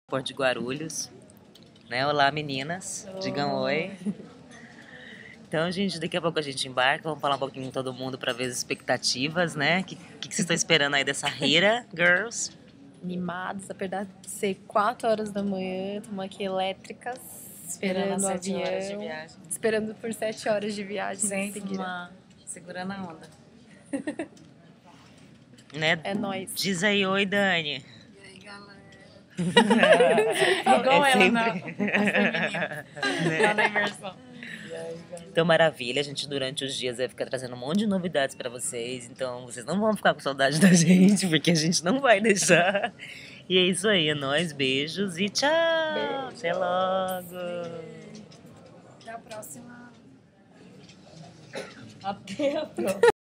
Gira Femininas 01 - Centroamérica
Ainda no Aeroporto de Guarulhos, Marielle Ramires, Carol Tokuyo, Lenissa Lenza e Dani Teixeira deixam o primeiro recado pros amigos no Brasil.

Este vídeo (e o aeroporto) são o Ponto de Partida da Gira das FEmininas pela cantroamérica, realizando a primeira Coluna de Sustentabilidade fora do país.